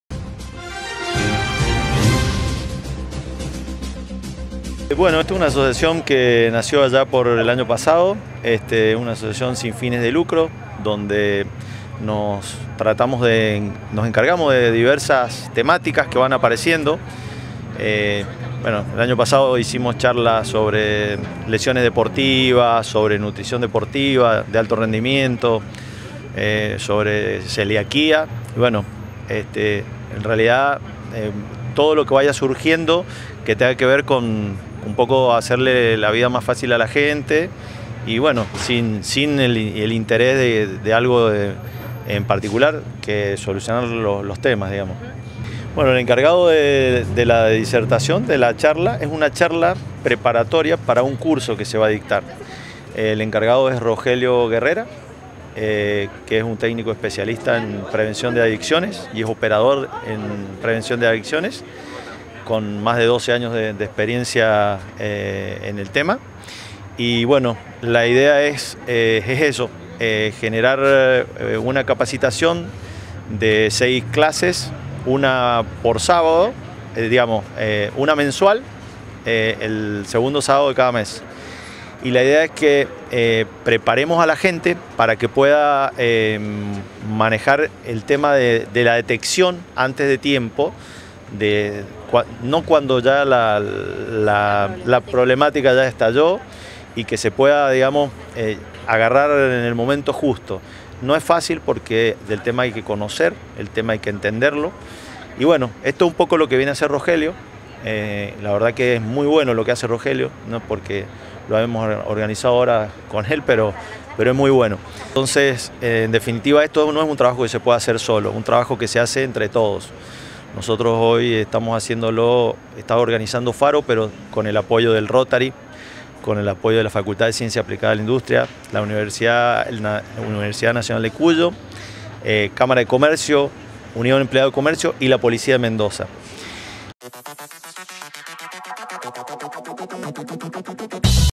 HACER LA VIDA MAS FÁCIL
La Asociación FARO está diagramando una charla preparatoria para generar una capacitación en la detección del consumo de drogas.